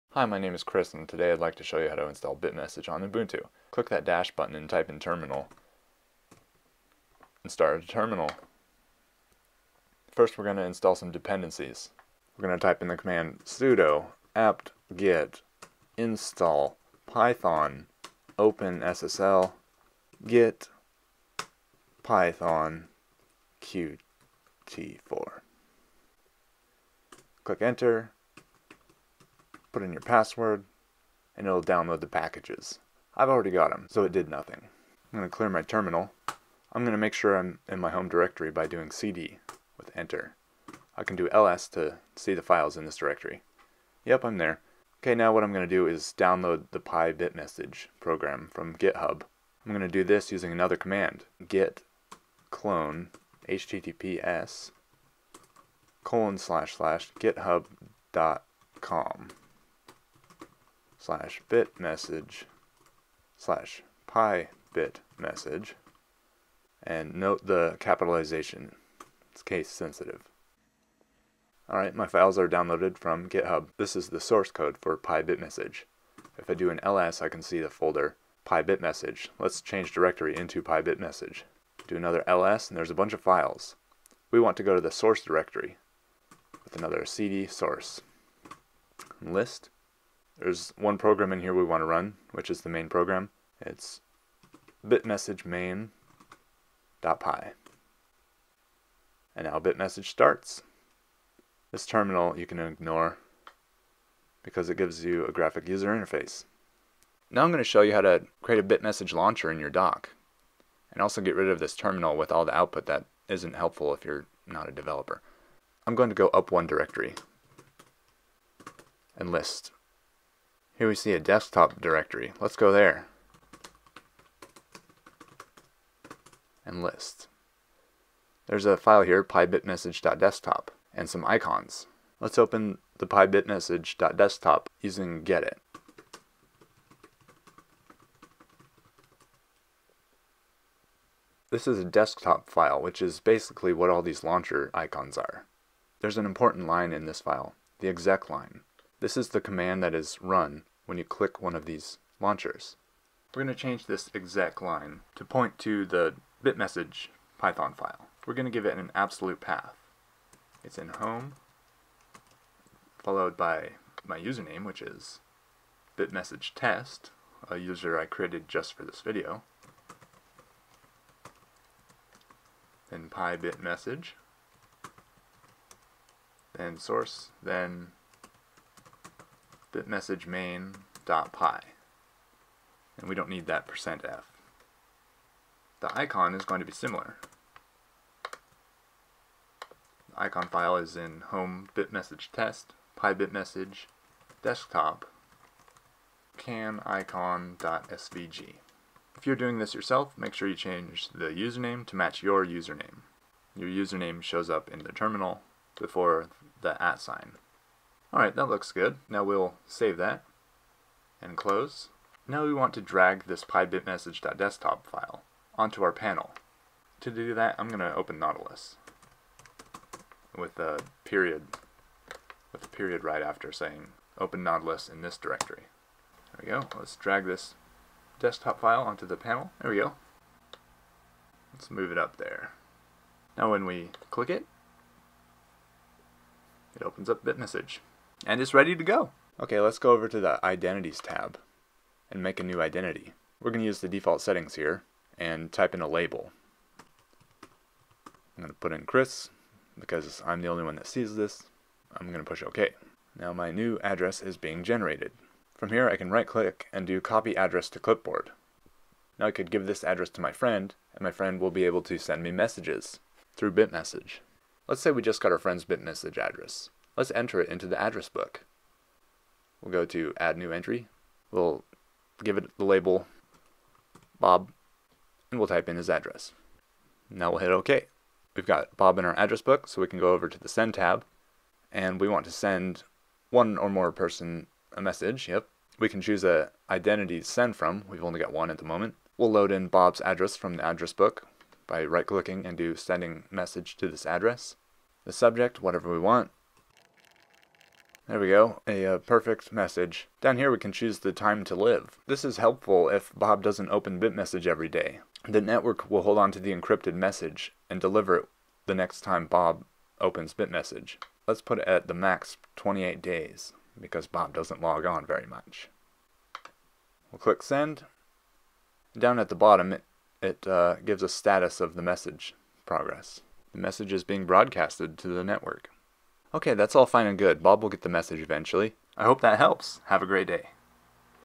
[tutorial] How to Install Bitmessage on Ubuntu
plus a segment on how to send messages to your friend

Want to get into Stock trading? Sign up with Robinhood and get a free stock